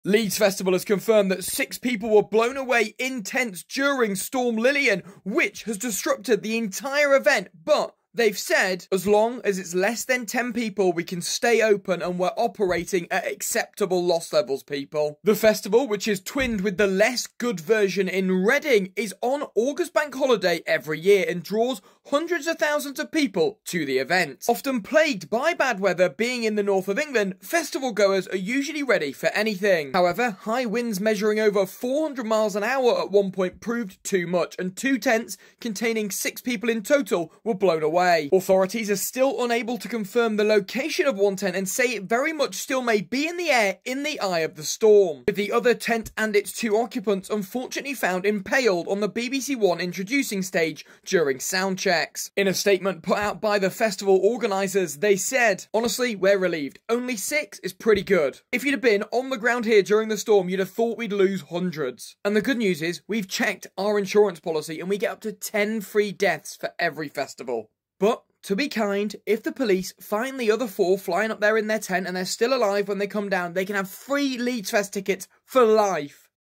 Storm Lilian Blows 6 People Away In Tents At Leeds Festival
A heavy storm has caused chaos at the festival with 6 people missing due to the windy weather

Leeds Festival was heavily impacted this year after multiple of it's stages were shut down due to the bad weather.